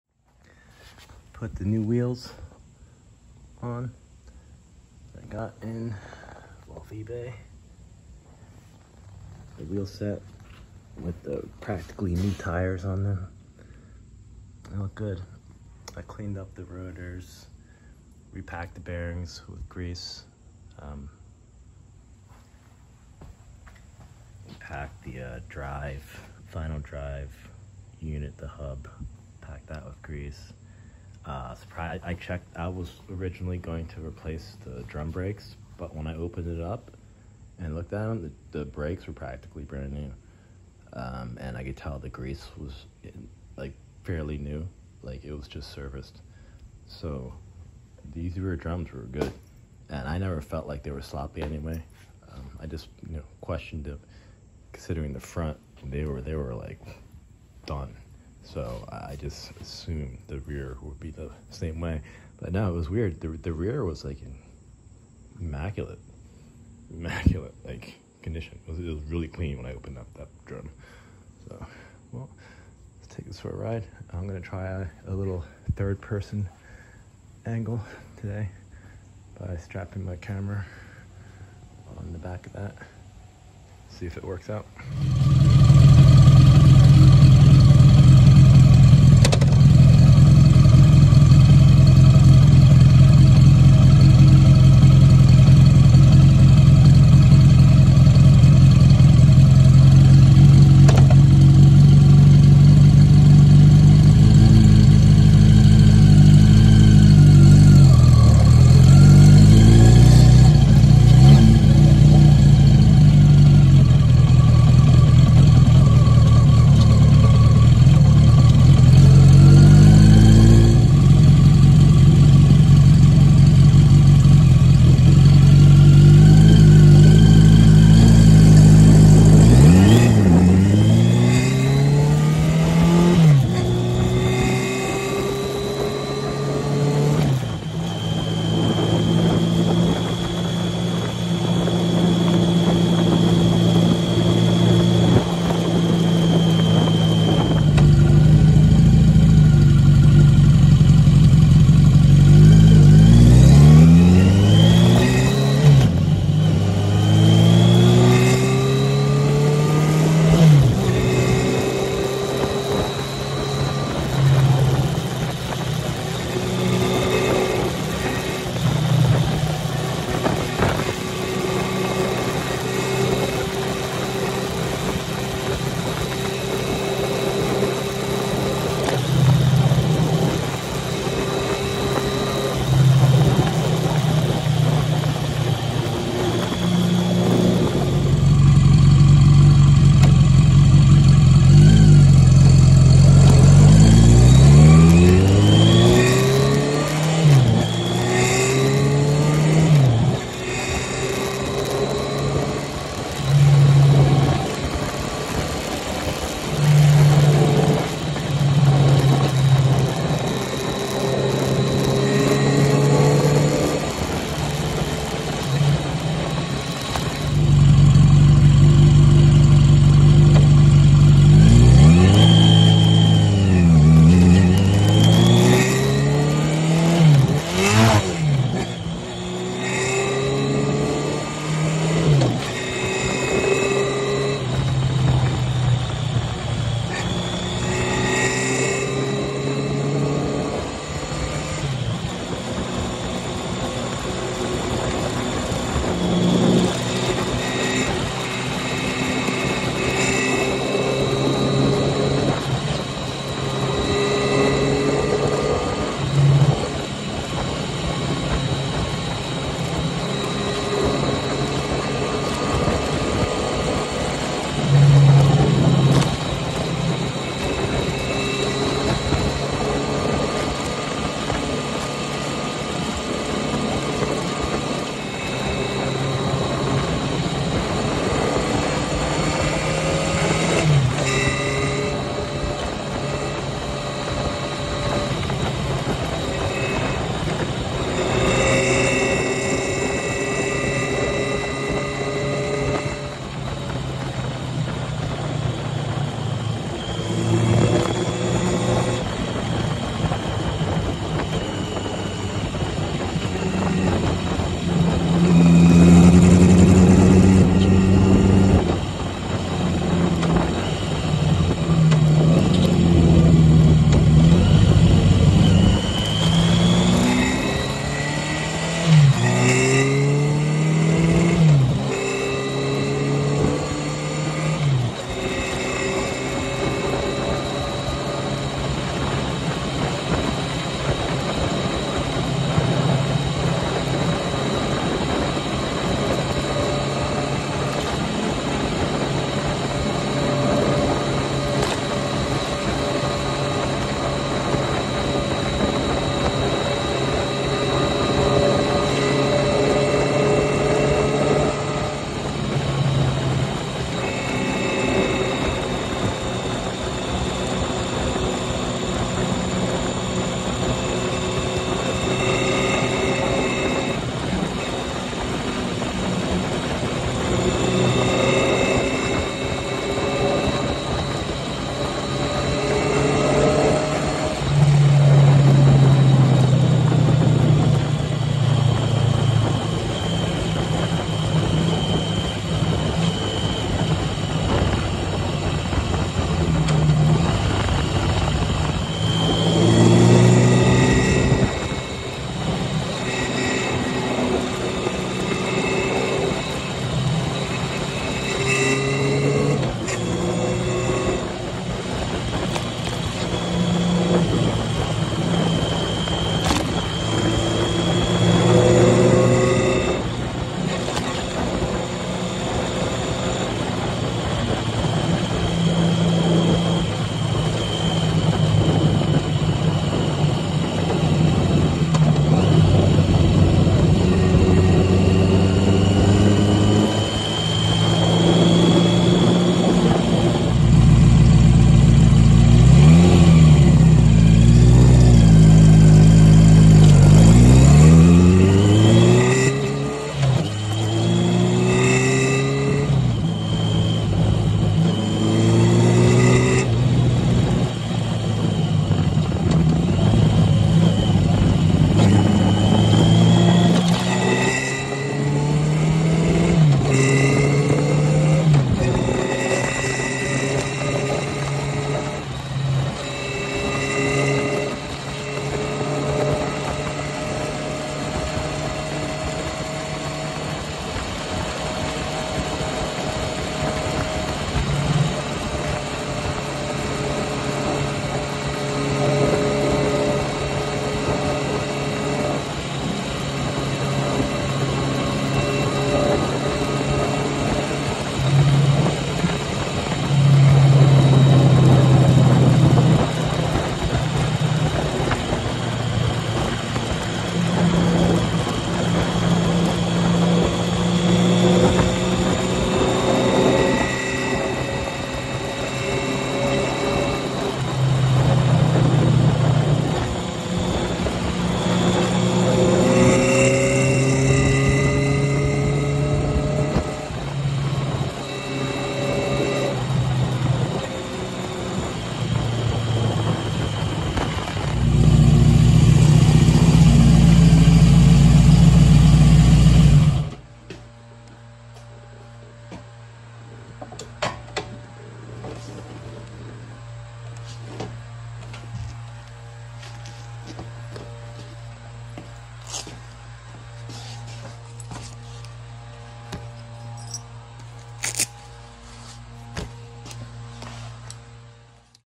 Yamaha XJ750 - Testing New Wheels
Trying out my new wheel set for the 1982 Yamaha XJ750 SECA.